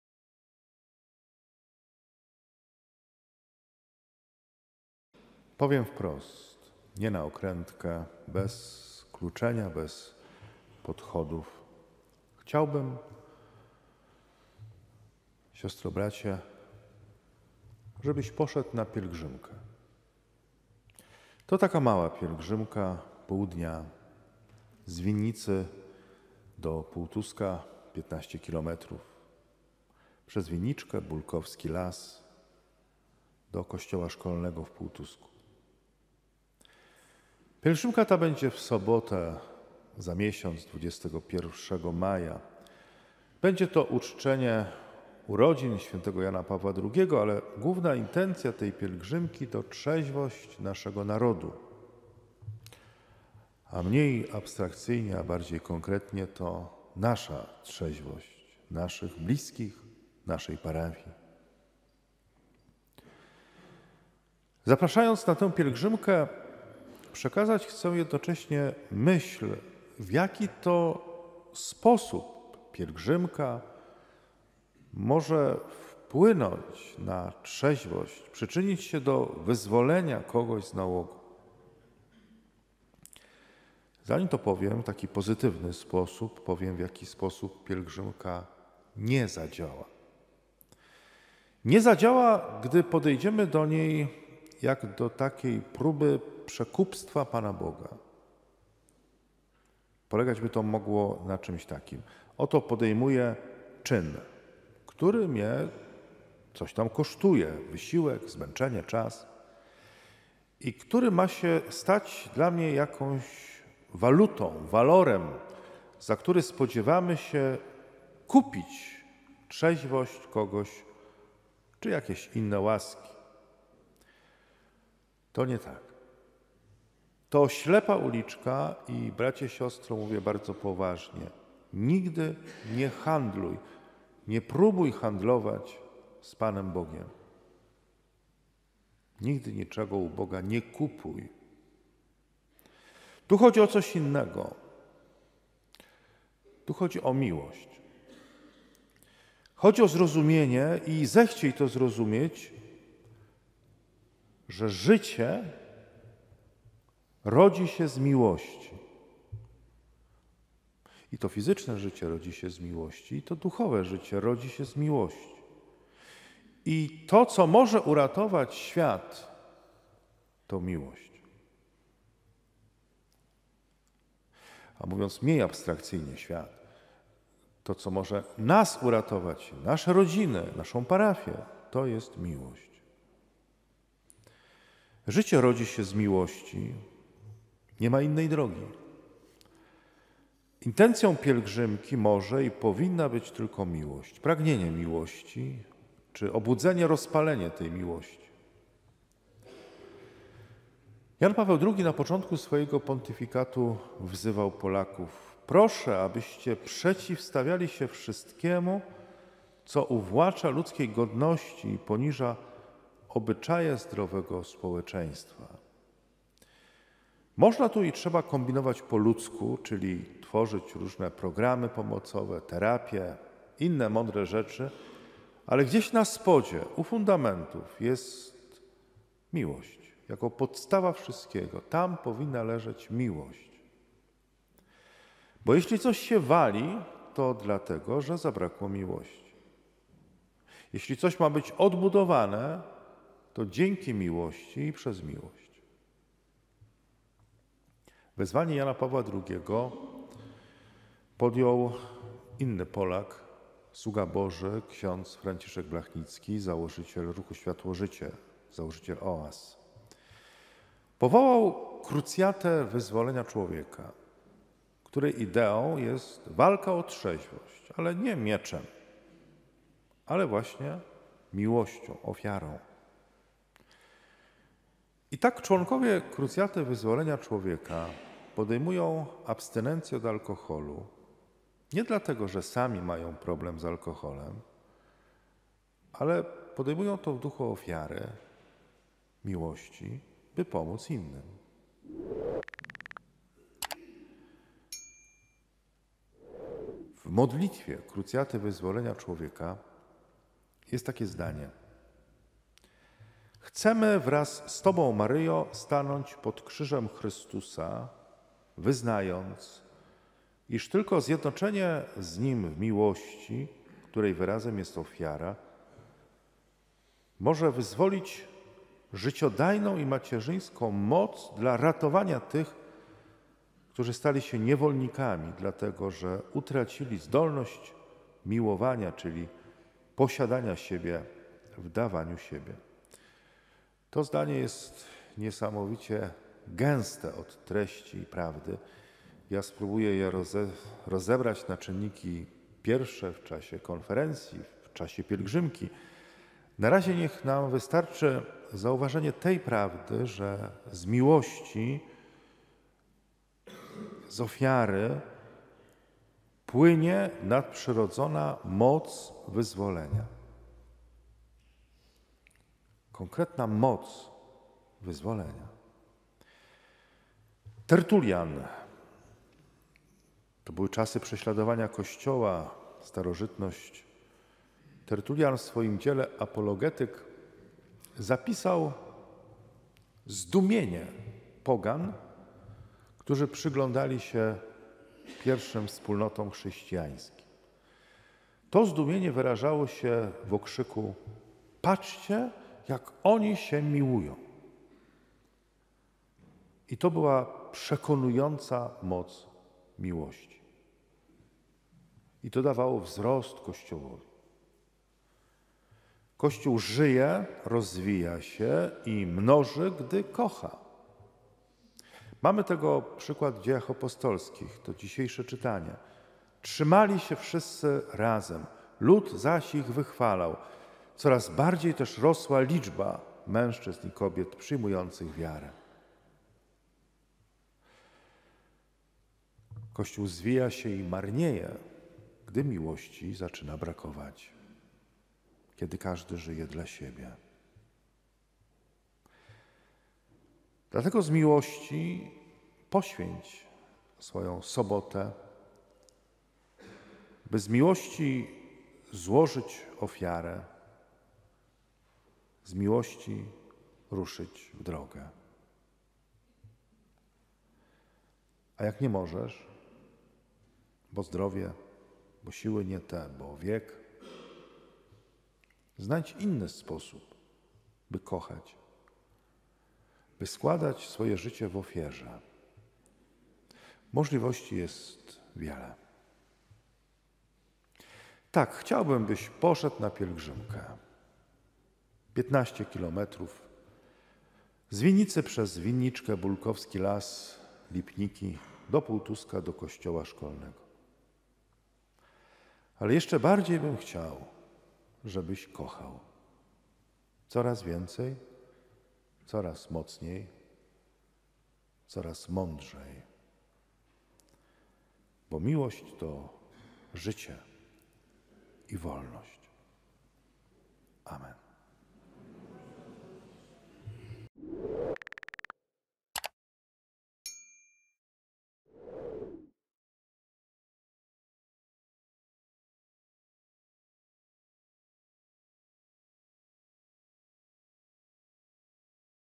🔴🔴 Życie i wolność - kazanie
Druga Niedziela Wielkanocna czyli Miłosierdzia Bożego; (Dz 5,12-16); (Ps 118,1 i 4,13-14.22 i 24); (Ap 1,9-11a.12-13.17-19); Aklamacja (J 20,29); (J 20,19-31); Winnica 24 kwietnia 2022.

Fundacja Nasza Winnica
Od Litery Do Wiary
Rzeczy Dobrze Pomyślane